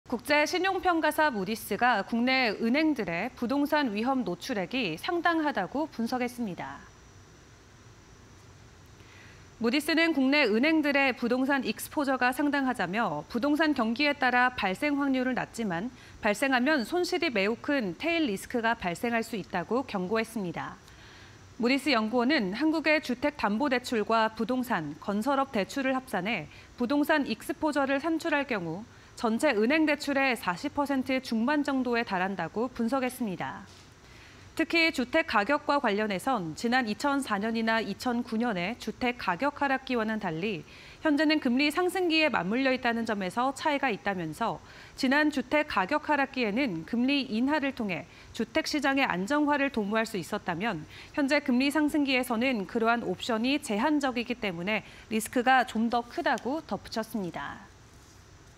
무디스 "한국 은행, 부동산 위험노출 상당... 리스크 현실화 우려" (20231122)
무디스 "한국 은행, 부동산 위험노출 상당... 리스크 현실화 우려"